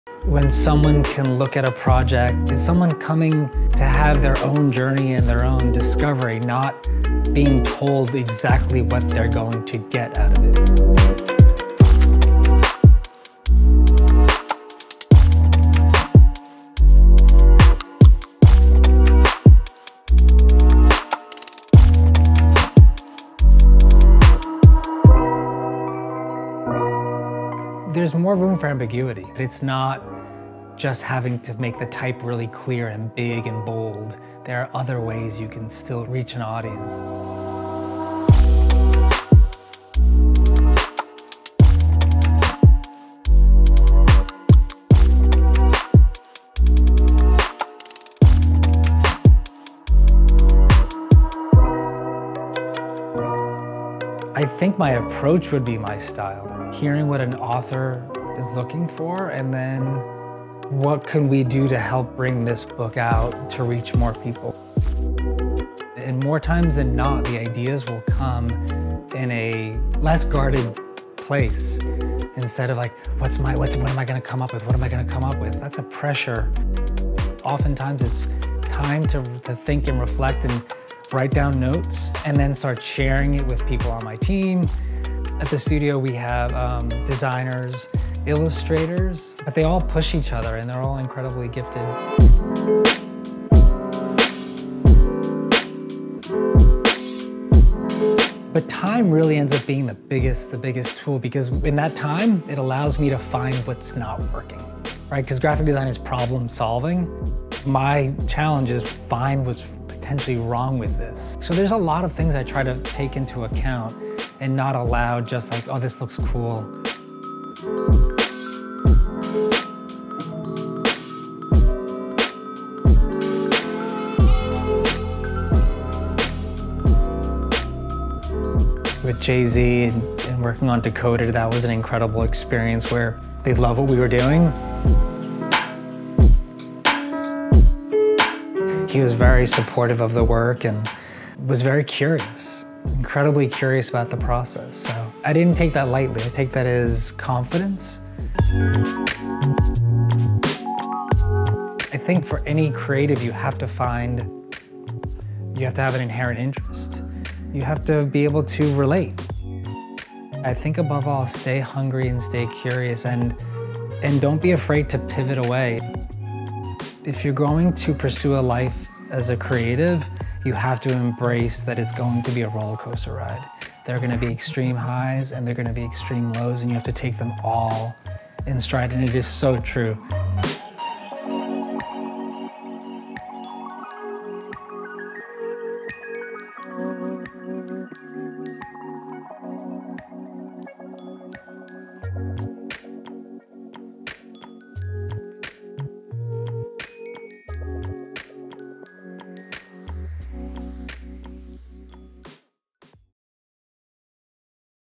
Domestika Creativos: Rodrigo Corral
Rodrigo Corral, artista gráfico estadounidense conocido por sus diseños de portadas de libros nos recibe en su estudio para repasar su carrera, hablar de su método de trabajo y de sus mejores proyectos.